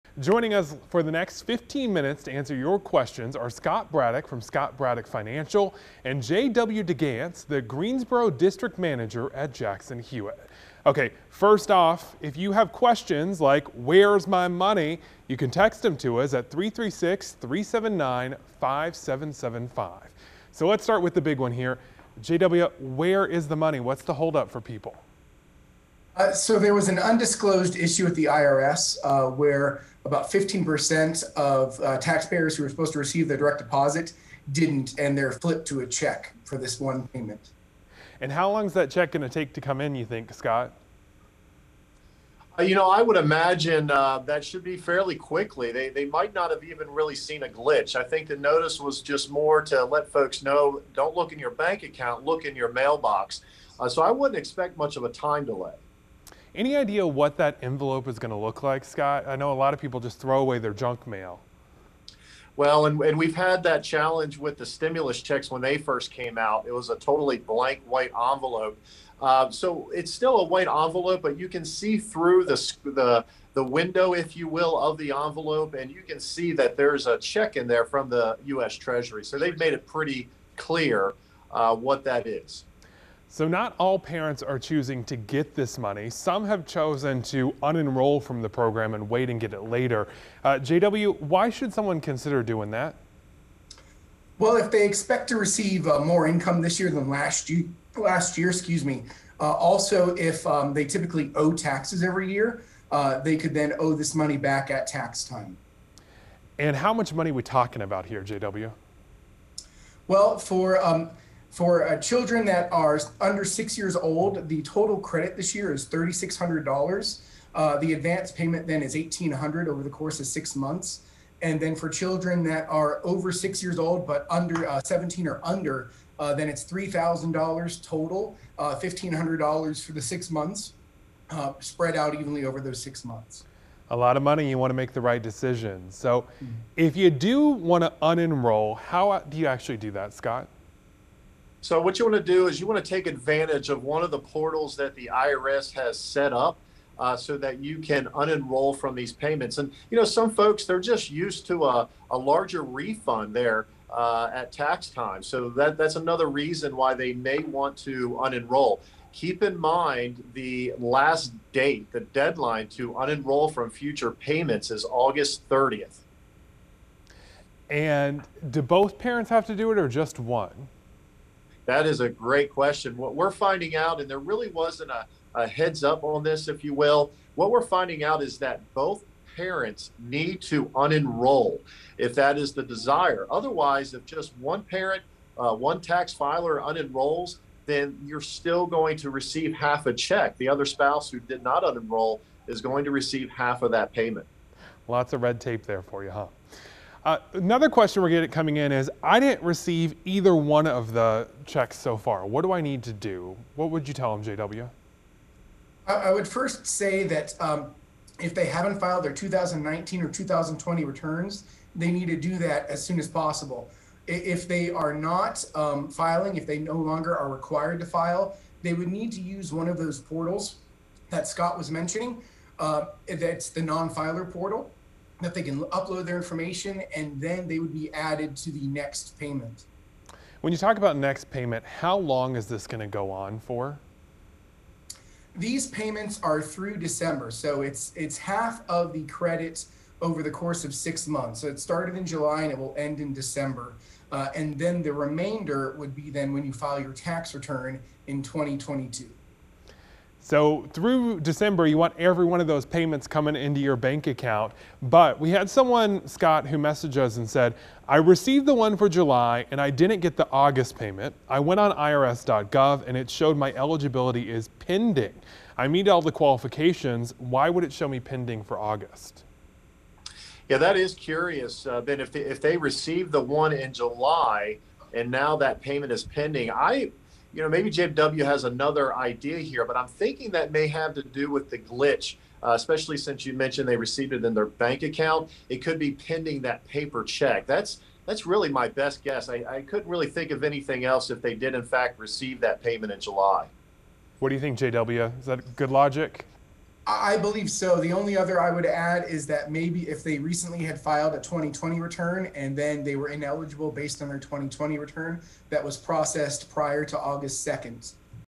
Answering your questions about the Child Tax Credit Payments: Part 1
The second round of Advanced Child Tax Credit payments went out on August 13, 2021.

The deadline to unenroll or opt out of the rest of the payments is at the end of this month. Just recently, the IRS also confirmed married couples must unenroll separately, or half payments will still be made.

The advance payments total about half the total child tax credit you will see on your 2021 taxes. If you like getting one big deduction at tax time, you’ll want to unenroll from the payments.

Also, if you're on the high end of the income threshold and you don't want the possibility of owing Uncle Sam, you may want to opt out. To unenroll you will use the Child Tax Credit Portal.